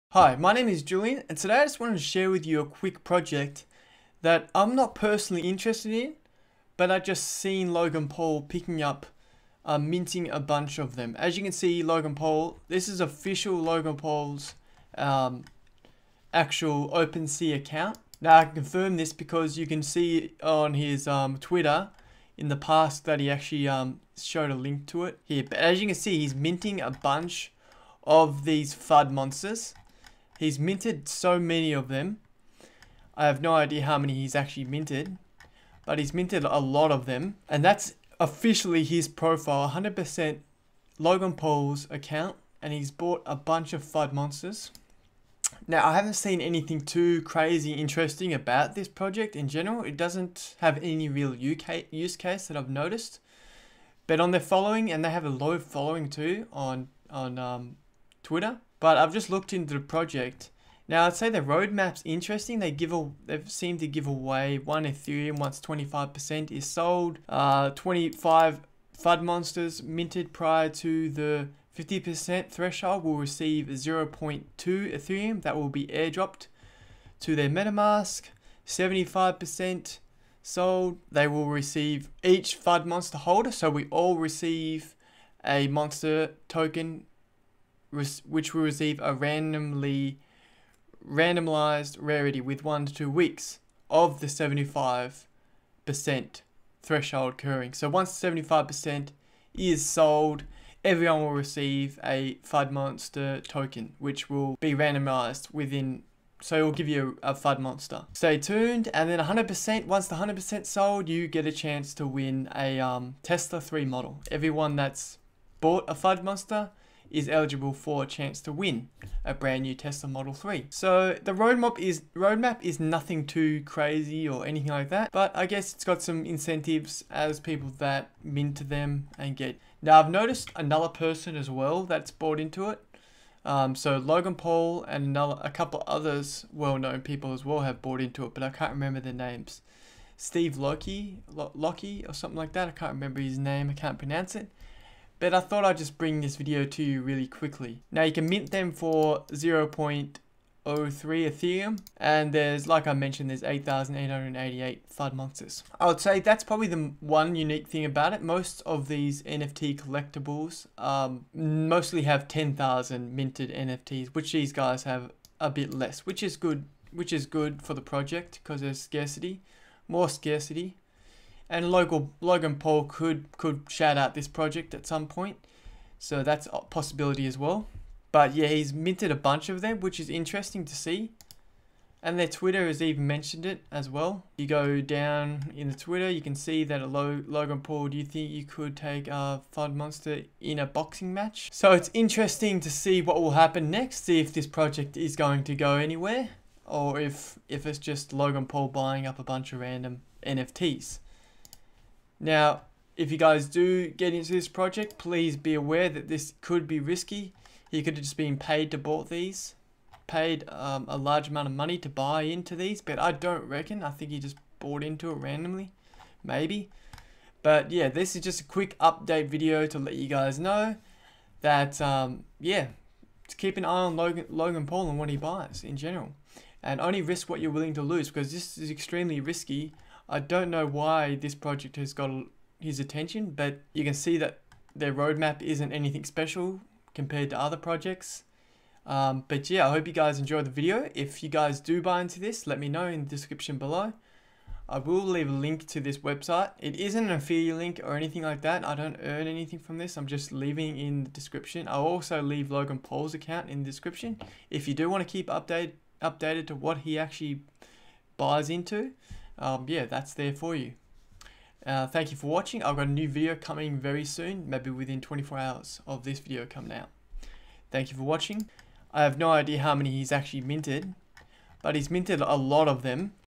Famous Influencer Buying The Next Big NFT (Maybe)
I just wanted to share with you a Famous influencer buying up a ton of these nft's and maybe this could be the next big nft maybe I'm not 100% sure I would risk only what you are willing to lose, now could this nft 10x, etc very possible if logan paul announces it to his followers, etc

Recommend Exchanges

Secure Your Cryptocurrency